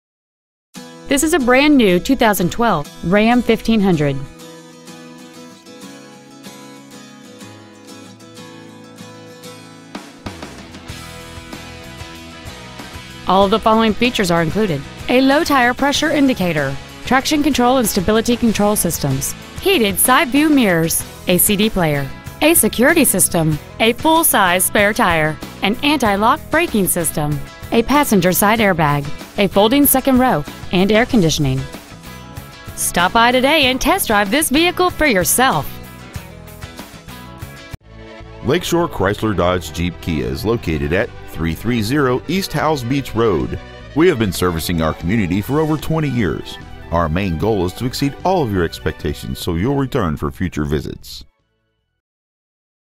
2012 Ram 1500 Slidell LA 70461
Year : 2012
 Make : Ram
 Model : 1500
 Engine : 4.7L V8 SOHC 16V FFV
 Trans . : Automatic
 Exterior : Sage Brush Pearl

 Interior : Dark Slate
 Stock : 44852

 330 E. Howze Beach Road

 2012 Ram 1500 Slidell LA